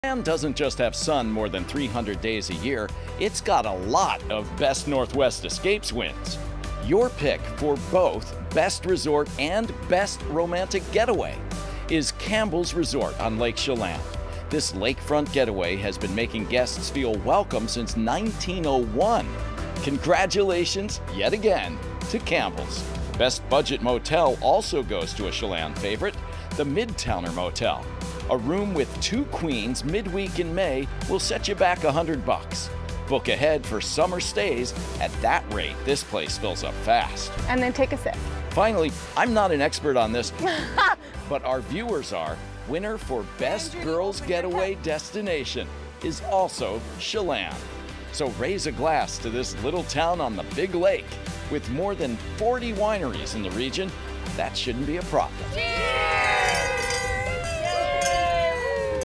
Eastern Washington getaway town a big winner - 2024's BNWE
Chelan, Campbell's Resort and Midtowner Motel all win in 2024's Best Northwest Escapes Viewer's Poll.